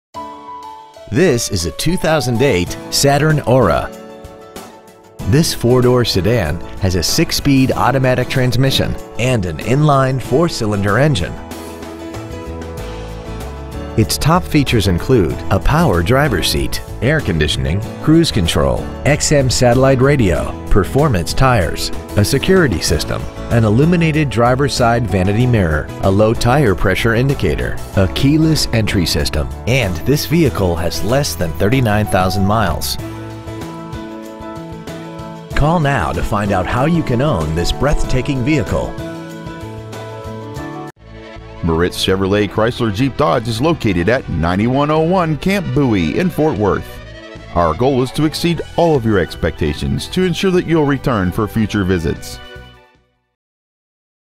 2008 Saturn Aura Dallas TX
Year: 2008
 Make: Saturn
 Model: Aura
 Engine: Gas I4 2.4L/145
 Trans.: Automatic
 Exterior: Silver Pearl
 Miles: 38,792
 Interior: Gray

 Used 2008 Saturn Aura Ft Worth TX